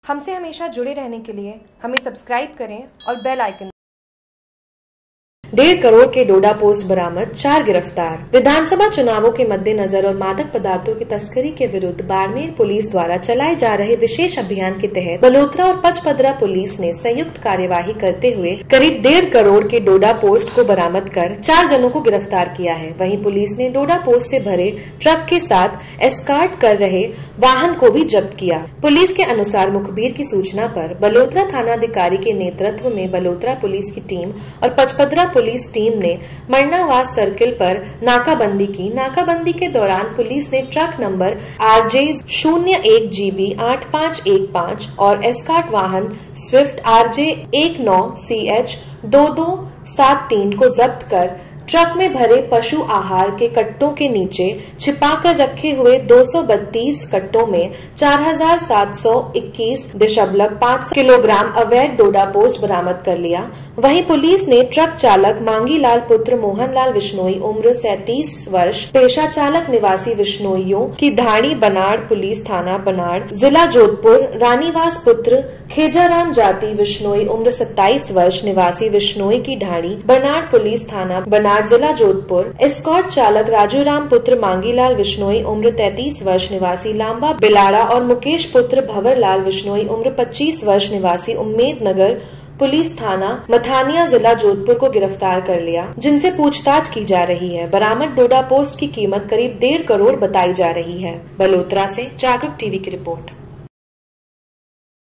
JAGRURK TV : बालोतरा : डेढ़ करोड़ के डोडा पोस्त बरामद, 4 गिरफ्तार
बालोतरा- विधानसभा चुनावों के मद्देनजर व मादक पदार्थों की तस्करी के विरुद्ध बाड़मेर पुलिस द्वारा चलाये जा रहे विशेष अभियान के तहत बालोतरा व पचपदरा पुलिस ने संयुक्त कार्यवाही करते हुए करीब डेढ़ करोड़ के डोडा पोस्त को बरामद कर 4 जनों को गिरफ्तार किया है। वहीं पुलिस ने डोडा पोस्त से भरे ट्रक के साथ एस्कॉर्ट कर रहे वाहन को भी जब्त किया है।

पुलिस के अनुसार मुखबीर की सूचना पर बालोतरा थानाधिकारी के नेतृत्व में बालोतरा पुलिस की टीम व पचपदरा पुलिस टीम ने मनणावास सर्किल पर नाकाबंदी की। नाकाबंदी के दौरान पुलिस ने ट्रक नंबर RJ 01 GB 8515 व एस्कॉर्ट वाहन स्विफ्ट RJ 19 CH 2273 को जब्त कर ट्रक में भरे पशु आहार के कट्टो के नीचे छिपाकर रखे कुल 232 कट्टो में 4721.500 किलोग्राम अवैध डोडा पोस्त बरामद कर लिया। वहीं पुलिस ने ट्रक चालक मांगीलाल पुत्र मोहनलाल विश्नोई उम्र 37 वर्ष पेशा चालक निवासी विश्नोईयों की ढाणी बनाड़ पुलिस थाना बनाड़ जिला जोधपुर, रामनिवास पुत्र खेराजराम जाति विश्नोई उम्र 27 वर्ष निवासी विश्नोईयों की ढाणी बनाड़ पुलिस थाना बनाड़ जिला जोधपुर, एस्कोर्ट चालक राजूराम पुत्र मांगीलाल विश्नोई उम्र 33 वर्ष निवासी लाम्बा बिलाड़ा व मुकेश पुत्र भंवरलाल विश्नोई उम्र 25 वर्ष निवासी उम्मेदनगर पुल्स थाना मथानिया जिला जोधपुर को गिरफ्तार कर लिया। जिनसे पूछताछ की जा रही है। बरामद डोडा पोस्त की कीमत करीब डेढ़ करोड़ बताई जा रही है।